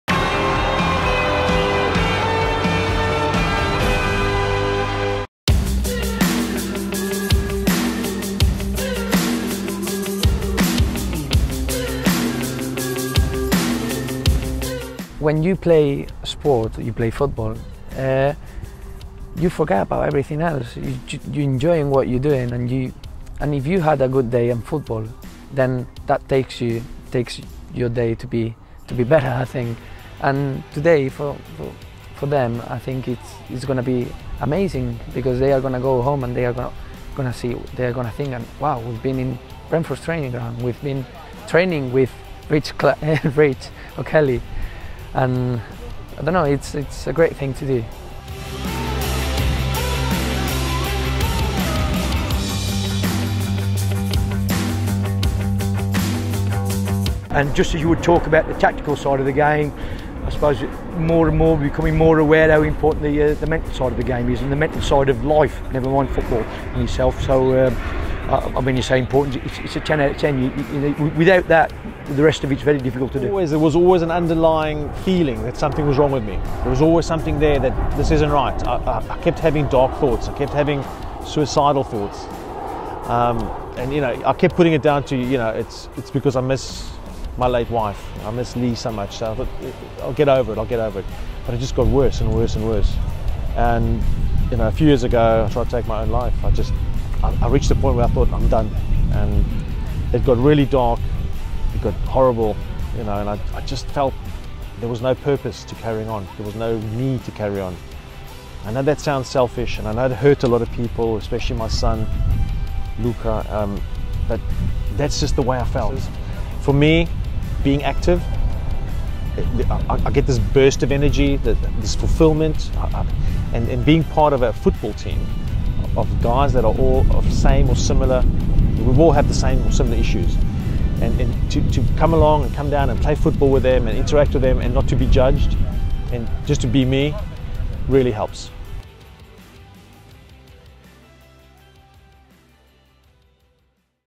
World Mental Health Day: Brentford - Richard O’Kelly and Sergi Canos
It was no ordinary training session for Brentford Assistant Coach Richard O’Kelly and winger Sergi Canos.

The duo ran a unique football coaching session for adults with mental health problems.